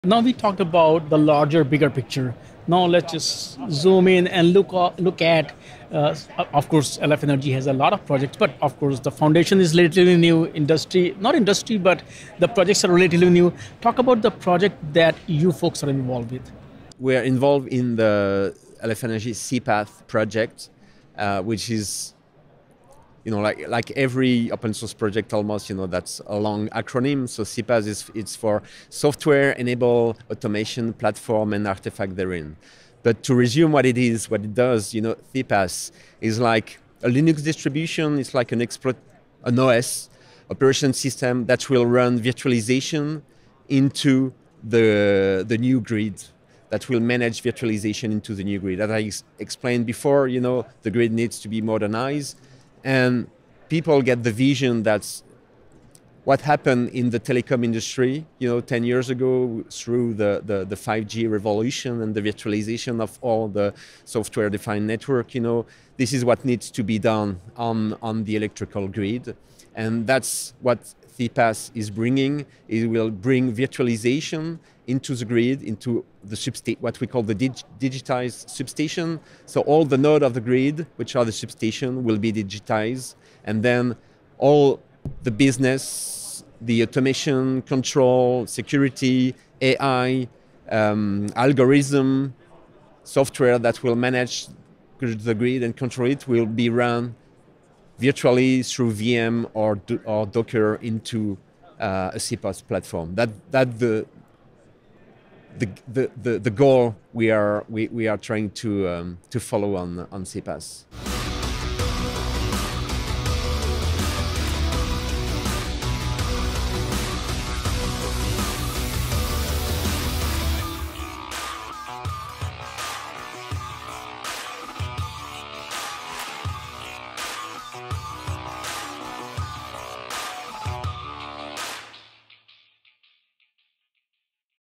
What is LF Energy SEAPATH’s initiative to modernize electrical grids? Christophe Villemer
In this clip, Christophe Villemer, Executive Vice President at Savoir-faire Linux, discusses their involvement with LF Energy’s Software Enabled Automation Platform and Artifacts (SEAPATH) project. He compares it to a Linux distribution for virtualization in modernizing electrical grids, similar to the telecom industry's shift to software-defined networks with 5G. He explains their goals to digitize substations and run automation, control, security, and AI algorithms virtually on a software platform within the grid.